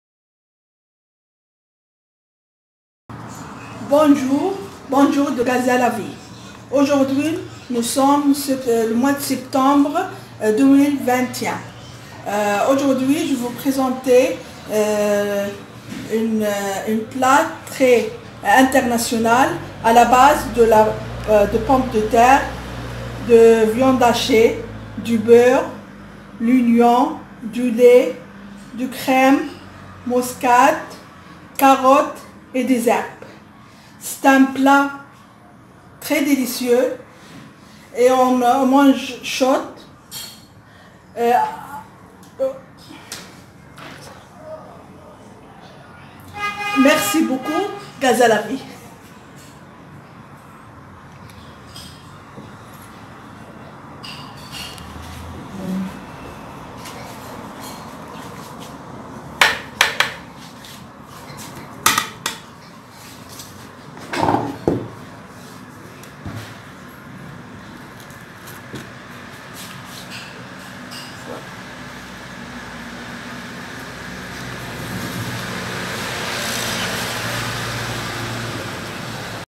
Le Hachis Parmentier. Un plat international à Gaza | Septembre 2021
Le Hachis Parmentier ce plat très délicieux est préparé à Gaza. Ce plat international à base de pommes de terre de viande hachée de beurre du lait de l'oignon des herbes et des carottes est préparé par Ghada une excellente cuisinière francophone de Gaza. Bravo. Bon appétit de Gaza la vie | Septembre 2021